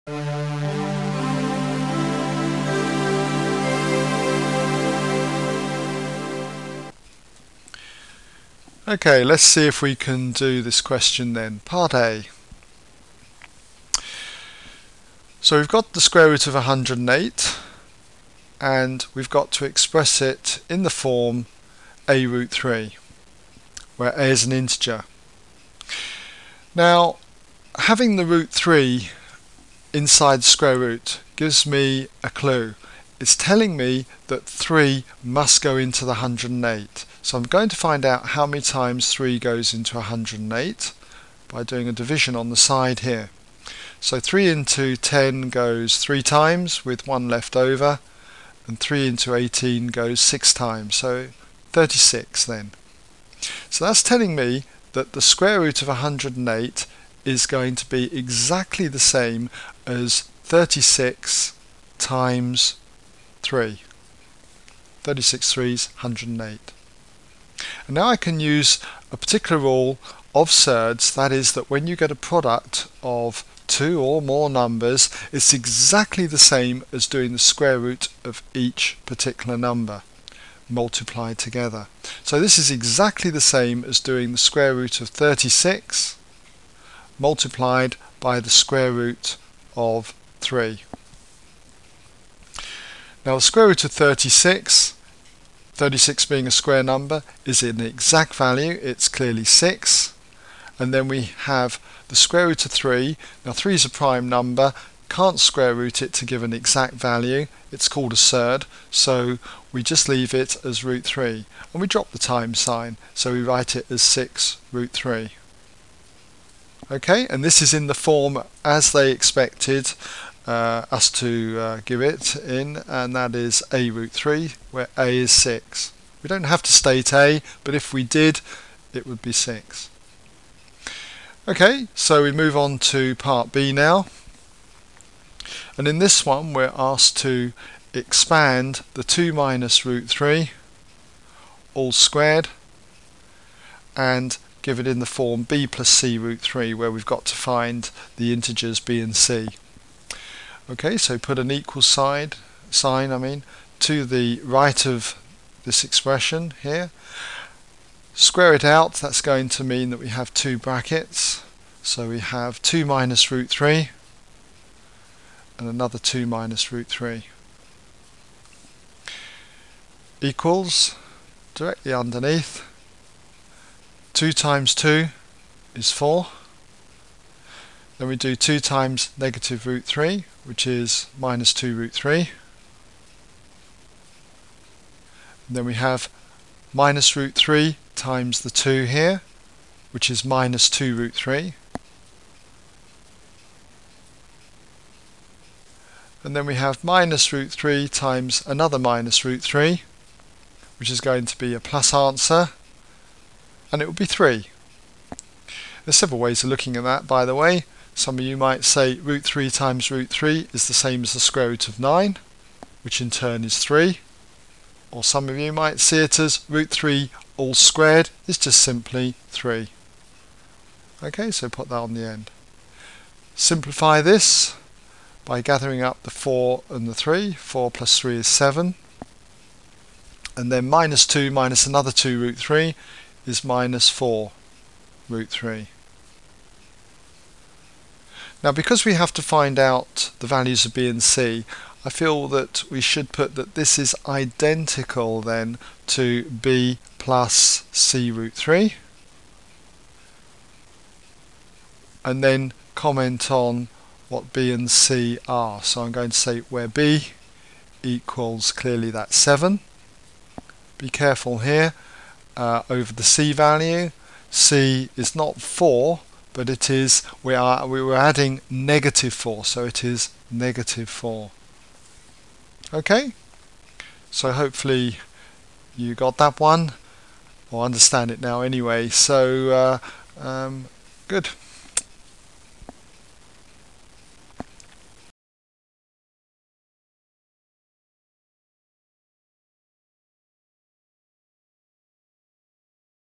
Edexcel C1 January 2007 Question 2 : ExamSolutions
Worked maths solution to this maths past paper question